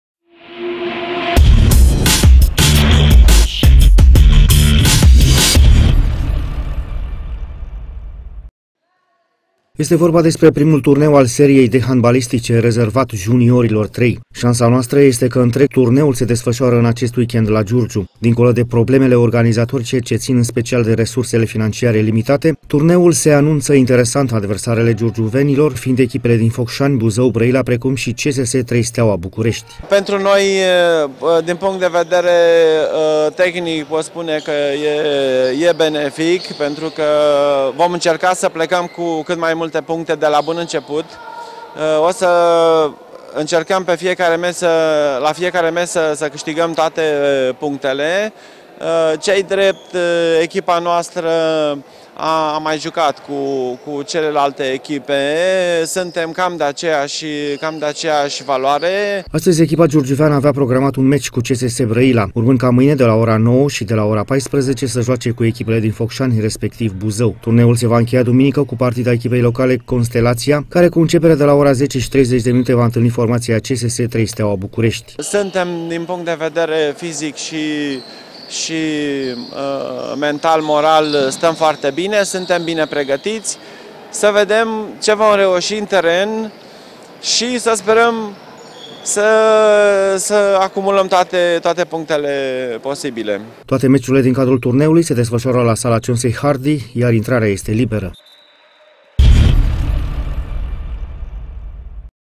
WEEK-END CU TURNEU HANDBALISTIC
Iubitorii de sport se pot bucură în acest week-end atât de handbal feminin cât și masculin. Handbaliștii de la echipa locală Constelația participă la Giurgiu la un turneu ce a început astăzi și se va încheia duminică.